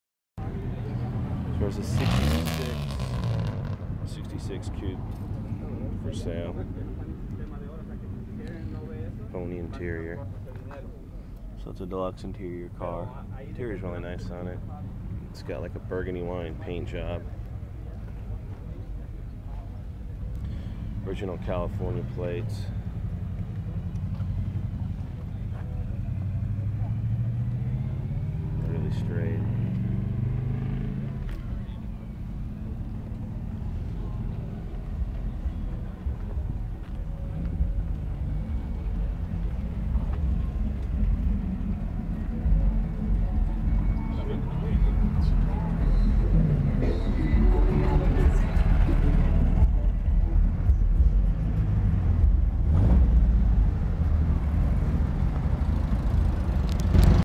1966 Ford Mustang 289 Pony Interior
1966 Ford Mustang 289 C code Auto, Pony interior, AC, Disc brakes.